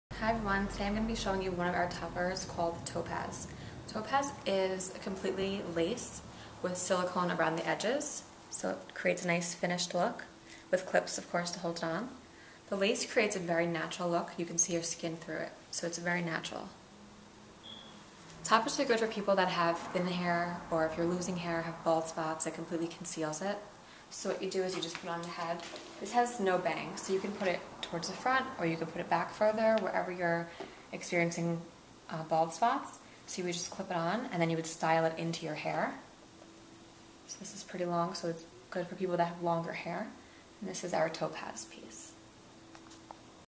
Milano Collection Wigs- Topaz Topper
Lace cap with silicone around the sides- no bangs.
Perfect alternative for patches of baldness or thinning hair. All toppers are meant to be blended in with natural hair. If you are experiencing full hair loss a full wig will be the ideal solution.

Weight: 70 grams
Length (front to back): 5
Width (right to left): 4 3/4